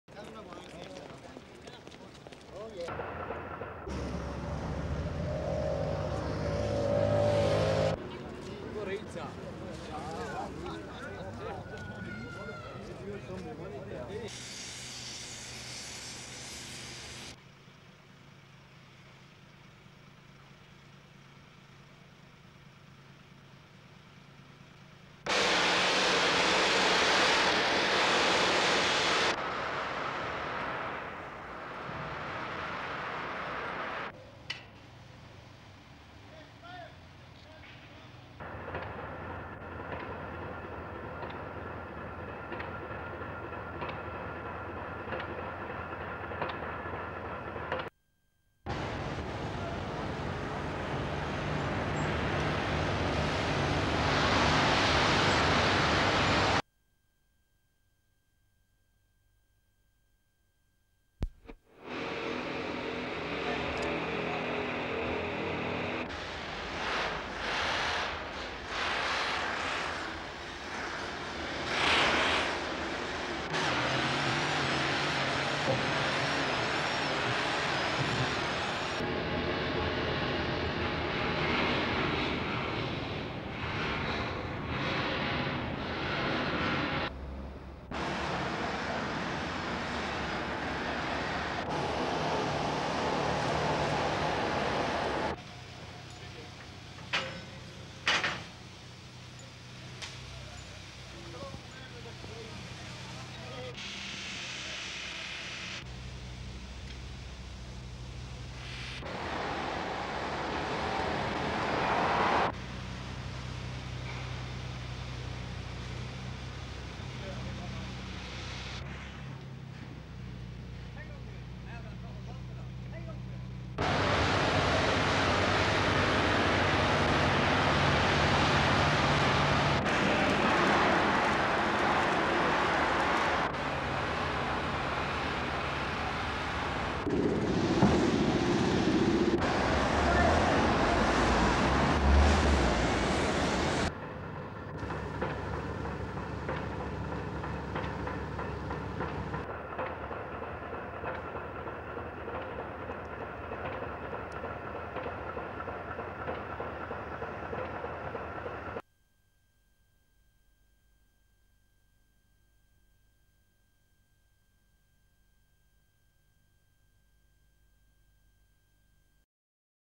Clyde Shipyard Work-In: Govan Shipbuilders Ltd
1970s - Men entering Govan Shipbuilders LTD and working on large ships.